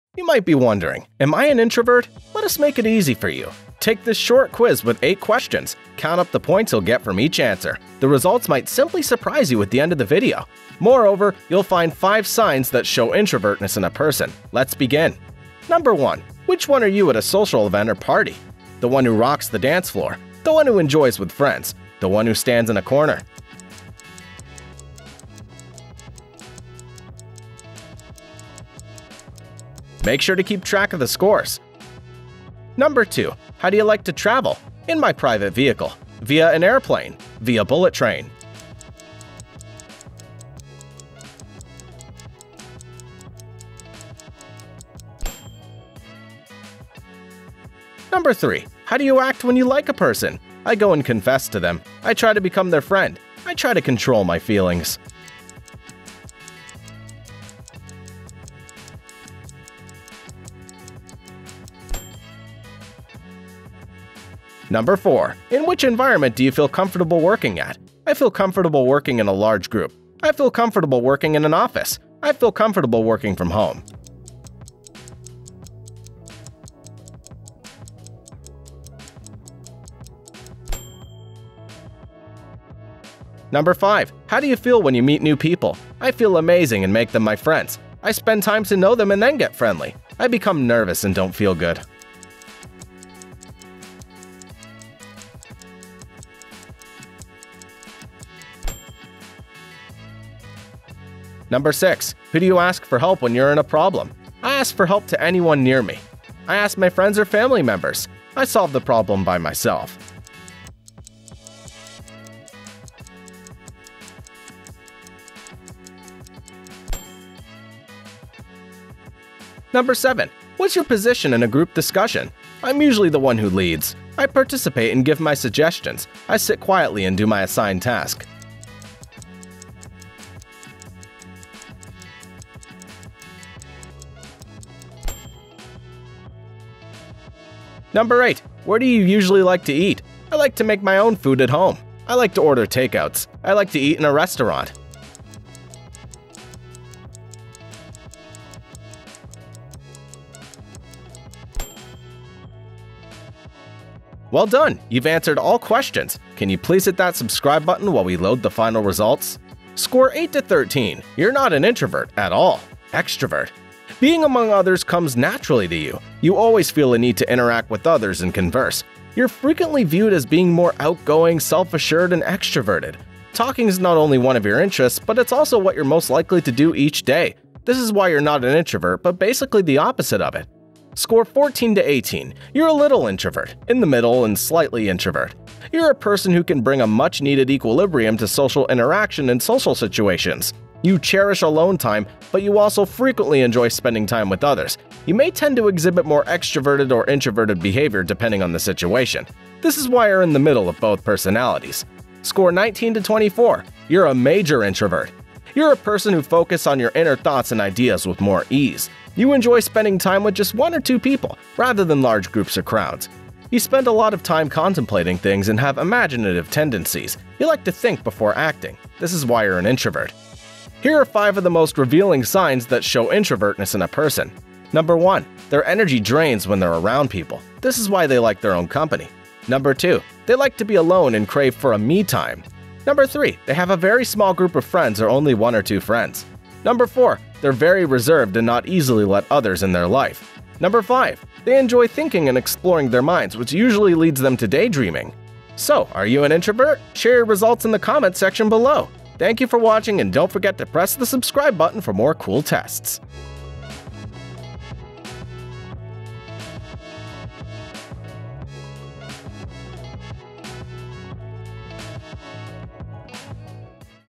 How INTROVERT Are You? Personality Test
You might be wondering, "Am I an introvert?" Let us make it easy for you.
Take this short quiz with 8 questions.
Count up the points you will get from each answer.
The results might simply surprise you at the end of video.
Moreover, you will find five signs that show someone is an introvert.

Please consider that this video is for fun and to provide information. It should not be used as a substitute for professional, medical, or psychiatric advice.